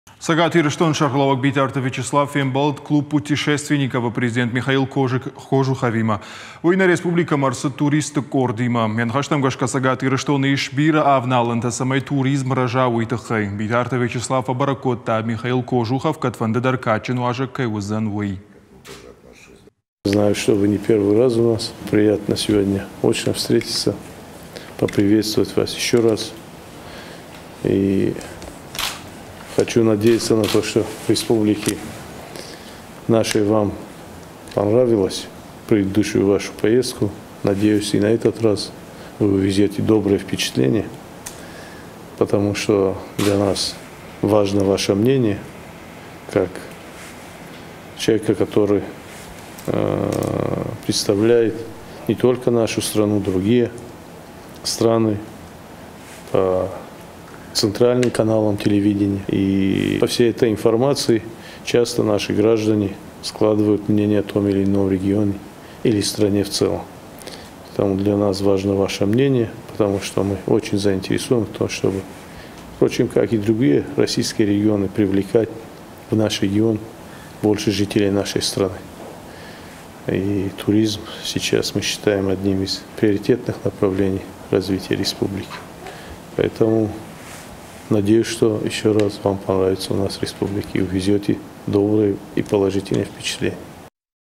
Цæгат Ирыстоны сæргълæууæг Битарты Вячеслав фембæлд «Клуб путешественников» ы президент Михаил Кожух
Цæгат Ирыстоны сæргълæууæг Битарты Вячеслав фембæлд «Клуб путешественников»-ы президент Михаил Кожуховимæ. Уый нæ республикæмæ æрцыд туристты къордимæ.

Йæ ныхæстæм гæсгæ, Цæгат Ирыстоны ис бирæ авналæнтæ, цæмæй туризм рæза, уый тыххæй. Битарты Вячеслав фæбæрæг кодта, Михаил Кожухов кæдфæндыдæр кадджын уазæг кæй уыдзæн, уый.
Мы рядом в:
ok.ru/irystontv